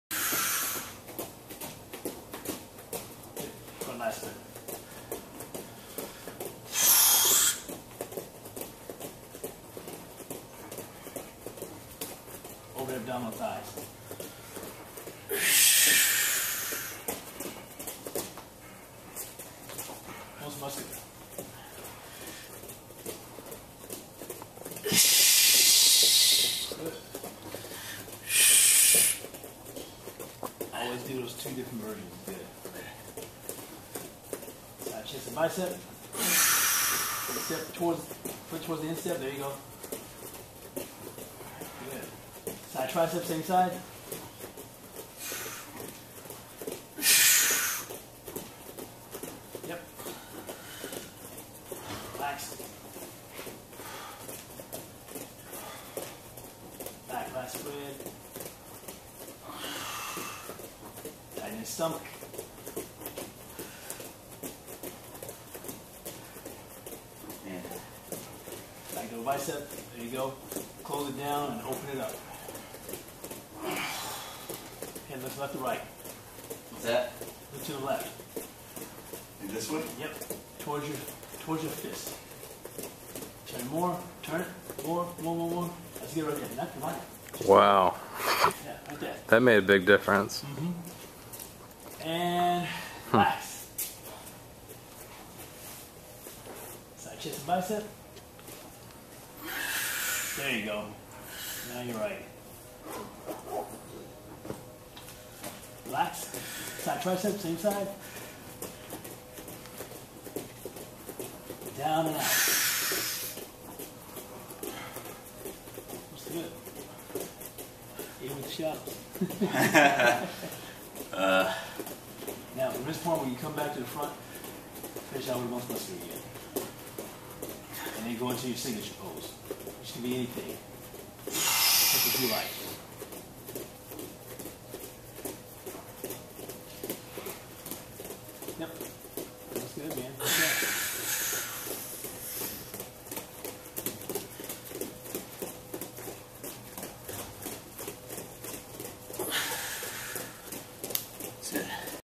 FIbromyalgia Bodybuilding Poses
This is me getting ready for the next show at muscle mania!